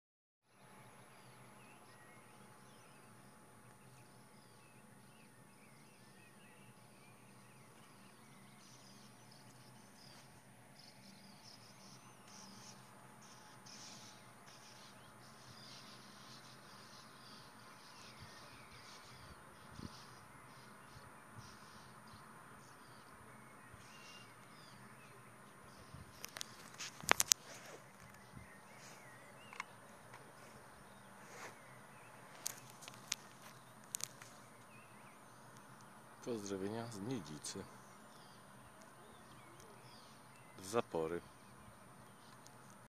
Zapora wodna w Niedzicy.. Zalew Czorsztyński 2018
Wycieczka zabytkową MZ 150  ... ;)